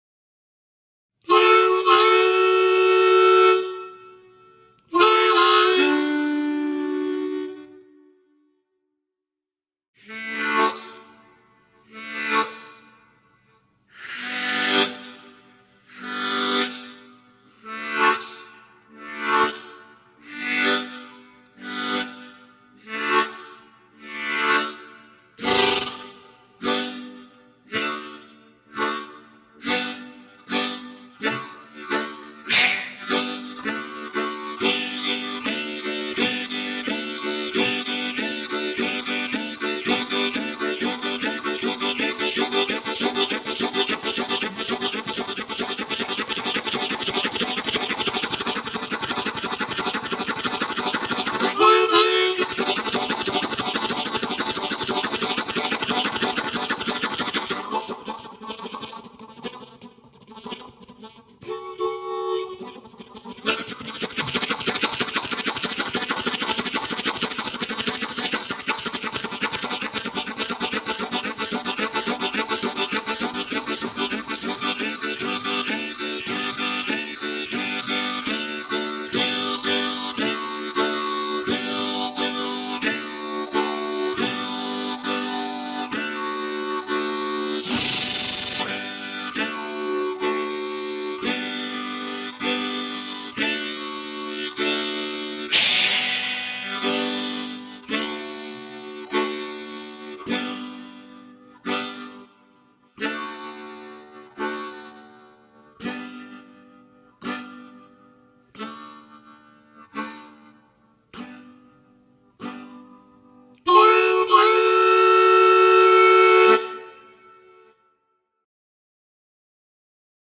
Imitazione del treno con l'armonica
Ecco come imitare il treno con l'armonica, un treno molto veloce!
Impariamo a riprodurre il fischio, il rumore del vapore, dei freni, e a far correre velocemente il nostro treno con l'armonica blues!